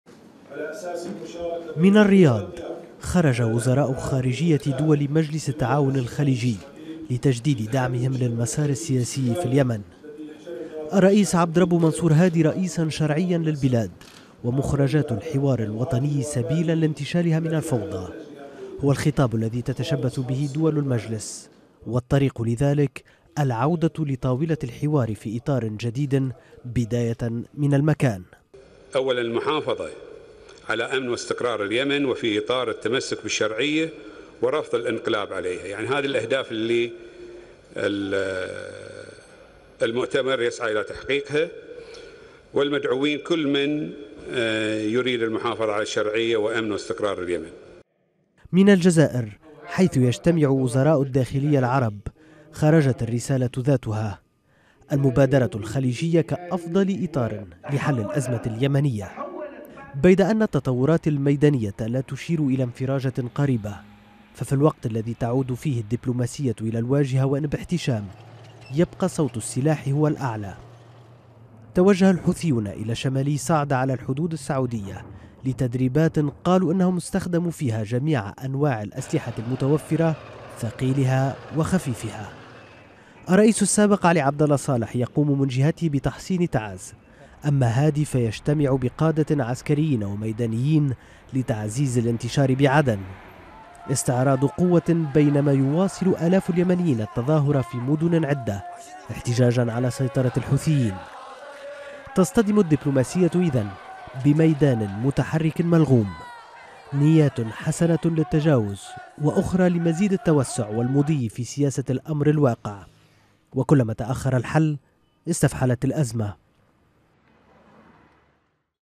اليمن.. حوار مطلوب ونزاع مفروض
ثلاثة أقطاب تتحرك عسكريا في اليمن، الحوثيون شمالا، في رسالة واضحة للجوار الخليجي، وعلي عبد الله صالح، في تعز في رسالة لغريمه الجديد وخليفته، عبد ربه منصور هادي، الذي يحشد في الجنوب أملا في استعادة شرعيته.